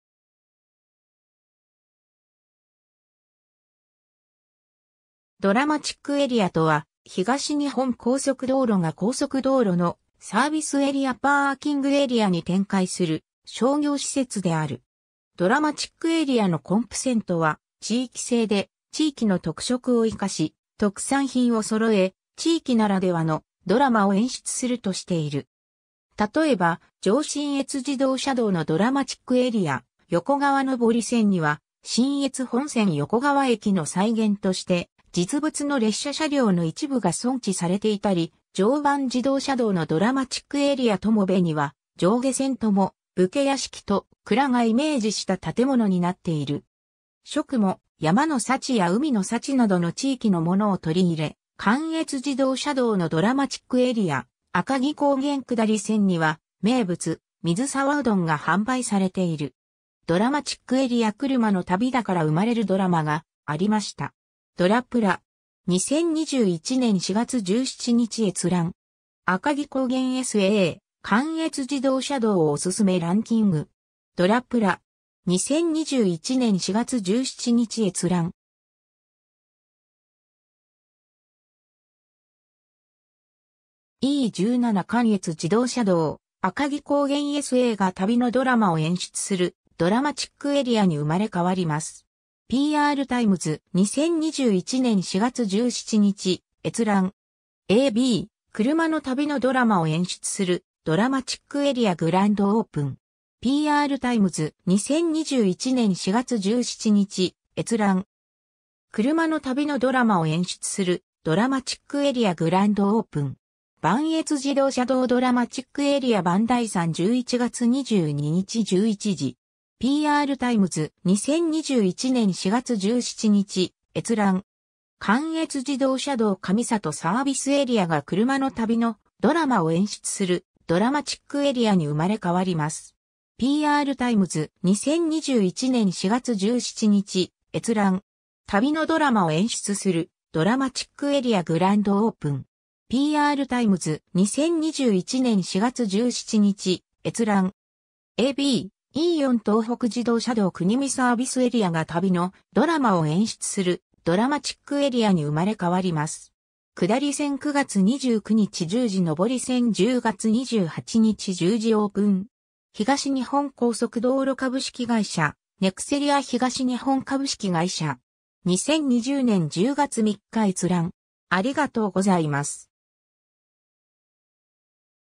ドラマチックエリア
日本のサービスエリア・パーキングエリア
ドラマチックエリアとは、東日本高速道路（以下「NEXCO東日本」）が高速道路のサービスエリア（SA）・パーキングエリア（PA）に展開する商業施設である。
 ドラマチックエリアのコンプセントは地域性で、地域の特色を生かし、特産品を揃え、地域ならではの「ドラマ」を演出するとしている。
 例えば、上信越自動車道のドラマチックエリア横川上り線には「信越本線横川駅」の再現として実物の列車車両の一部が存置されていたり、常磐自動車道のドラマチックエリア友部には上下線とも「武家屋敷」と「蔵」がイメージした建物になっている。
 食も、山の幸や海の幸などの地域のものを取り入れ、関越自動車道のドラマチックエリア赤城高原下り線には名物「水沢うどん」が販売されている。
 ^ “ドラマチックエリア クルマの旅だから生まれるドラマがありました”. ドラぷら. 2021年4月17日閲覧。
 ^ “」赤城高原SA(下)・関越自動車道 おすすめランキング”. ドラぷら. 2021年4月17日閲覧。
 ^ “E17関越自動車道 『赤城高原ＳＡ（上り線）』が旅のドラマを演出するドラマチックエリアに生まれ変わります！”. PR TIMES. 2021年4月17日閲覧。
 ^ a b “クルマの旅のドラマを演出する「ドラマチックエリア」グランドオープン！”. PR TIMES. 2021年4月17日閲覧。
 ^ “クルマの旅のドラマを演出する「ドラマチックエリア」グランドオープン！ 磐越自動車道　ドラマチックエリア磐梯山（下り線） １１月２２日（土）　１１時”. PR TIMES. 2021年4月17日閲覧。
 ^ “関越自動車道 上里サービスエリア（上り線）がクルマの旅のドラマを演出するドラマチックエリアに生まれ変わります！”. PR TIMES. 2021年4月17日閲覧。
 ^ “旅のドラマを演出する「ドラマチックエリア」グランドオープン！”. PR TIMES. 2021年4月17日閲覧。
 ^ a b “E4東北自動車道 国見サービスエリア（上下線）が旅のドラマを演出するドラマチックエリアに生まれ変わります！ 〜下り線9月29日（火）10:00 上り線10月28日（水）10:00 オープン〜 (PDF)”. 東日本高速道路株式会社・ネクセリア東日本株式会社 (2020年9月16日). 2020年10月3日閲覧。